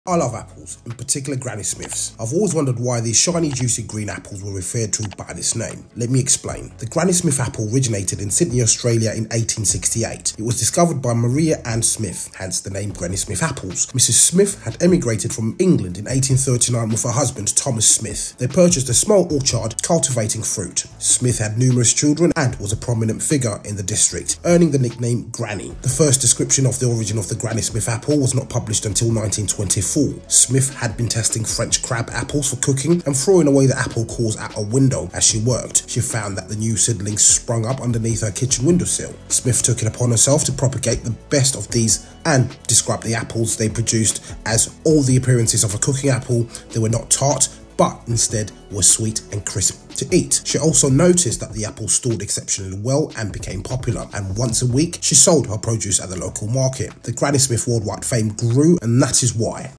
Origins Of Granny Smith Apples 🍏
The 'Granny Smith' apple originated in Sydney Australia, in 1868. It was discovered by Maria Ann Smith, hence the name Granny Smith apples.